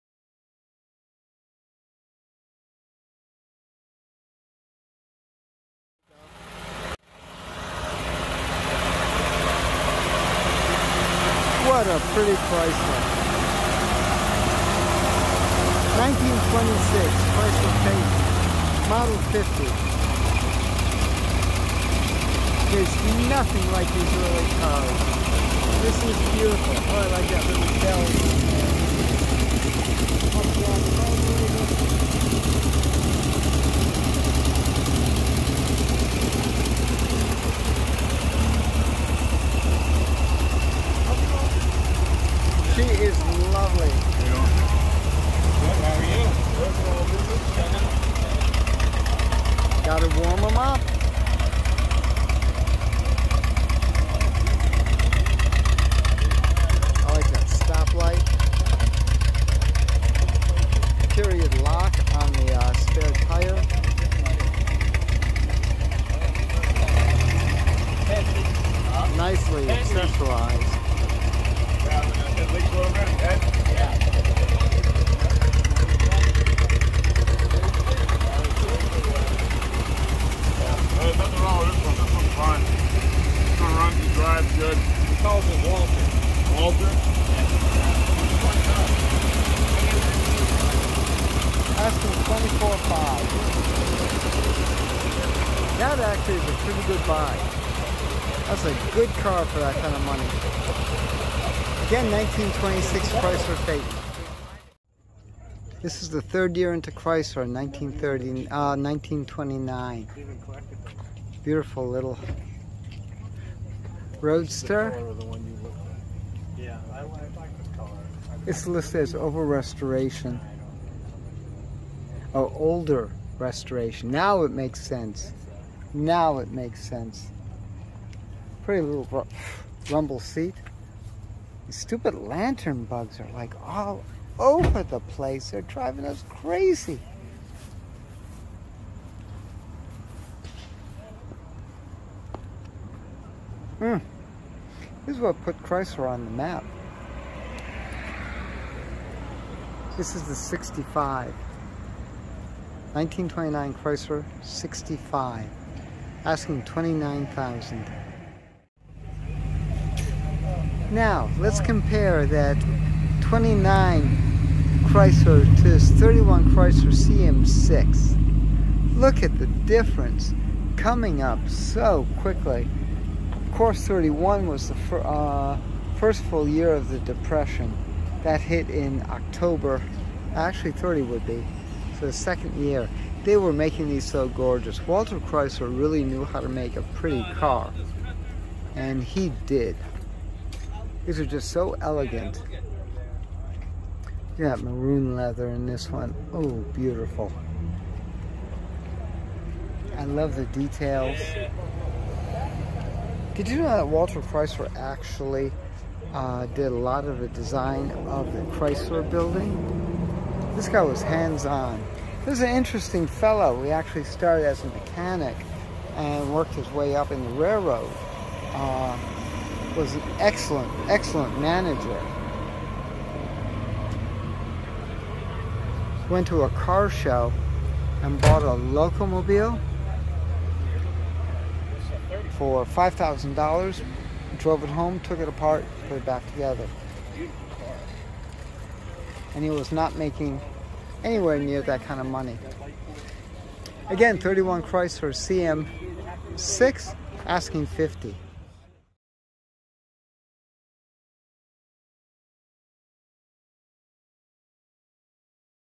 3 Prewar Chrysler s in the 2021 Hershey Swap Meet Car Corral
See the evolution early Chrysler in these, 3 Chryslers offered for sale in the 2021 Hershey swap meet car corral. 1926 Chrysler F58, 1929 Chrysler Model 65 and 1931 Chrysler CM For more photos, videos and your regional calendar of events,